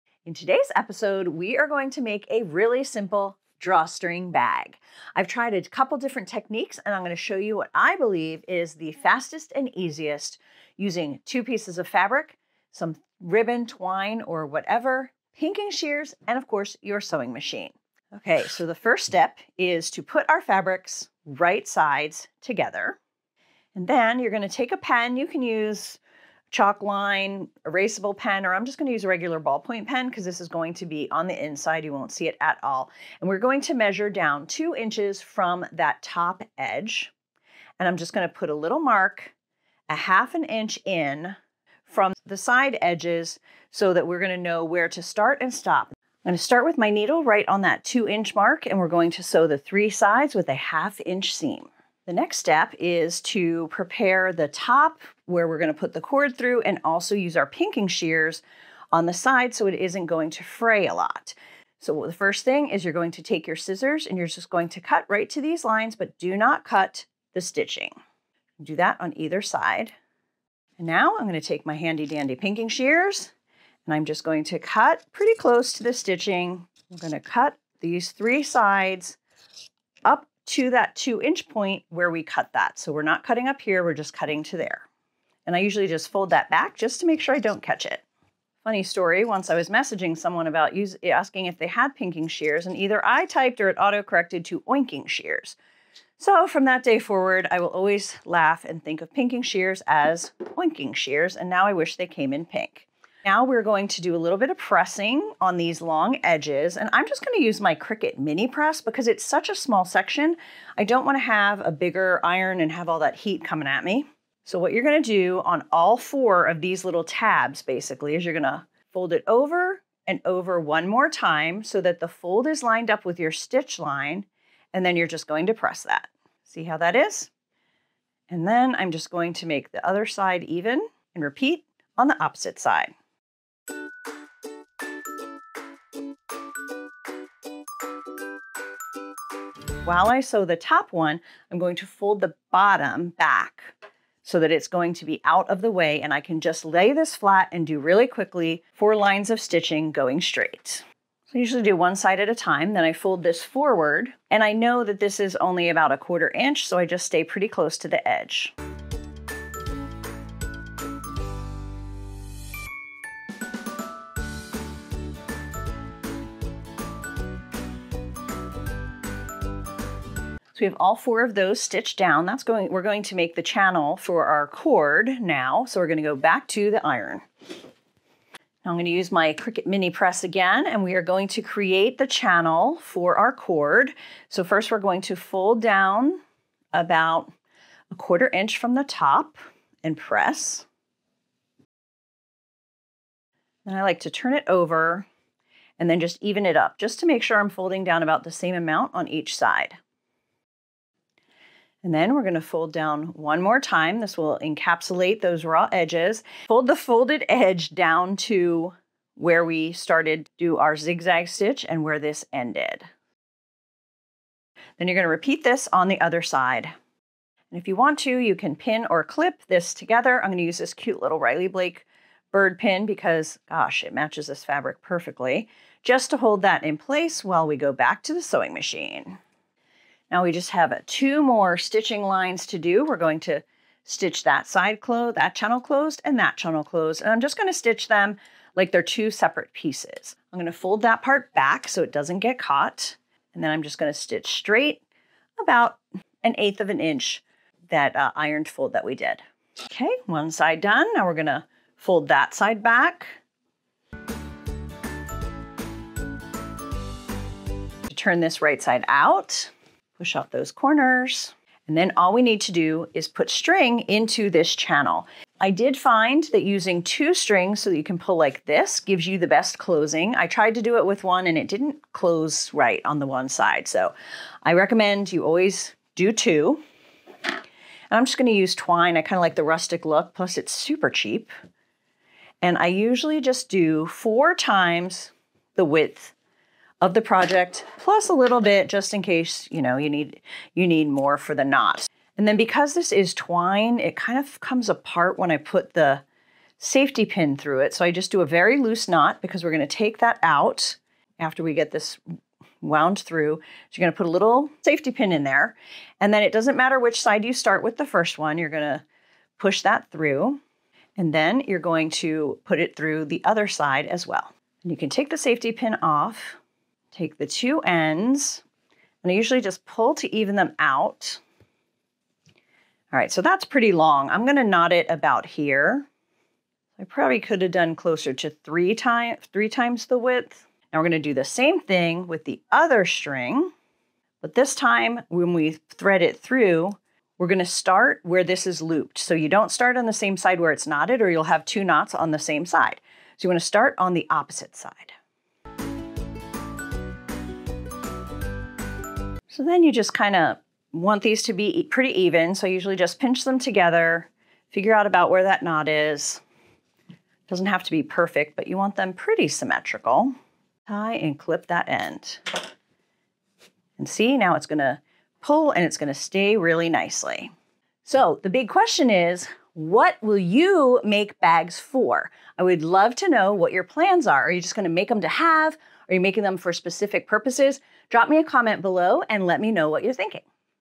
🛍️ How to Sew a Cute and Simple Drawstring Bag from Fabric Scraps
In this tutorial, I'll show you the easiest way to make a simple drawstring bags from your fabric scraps. This project is perfect for beginners, and you'll be amazed at how quickly you can whip up these adorable bags. 💡 Don't miss why to use 2 strings at 4 mins 29 seconds.

🧵 LOVE TO SEW?

These DIY drawstring bags are not only practical but also make fantastic handmade gifts or gift bags that the recipient can use. Whether you're an experienced sewer looking for a quick project or a beginner eager to learn, this tutorial is perfect for you.

This tutorial features my Happy at Home fabric available from Riley Blake Designs.

So, grab your fabric scraps, sewing machine, and enthusiasm, and let's get started on this fun and rewarding sewing adventure. Happy sewing!

🛒 AMAZON LINKS TO PRODUCTS USED IN THIS VIDEO
✅ Pinking Shears
✅ Cricut EasyPress Mini

🎯 SHORTCUTS TO SPECIFIC TOPICS:
00:00 introduction
00:12 Drawstring Bag Supplies
00:19 Prep the fabric
00:46 Sew fabrics
00:56 Prep drawstring channel
04:01 Sew drawstring channel
04:25 Add string to your bag
05:36 Add second drawstring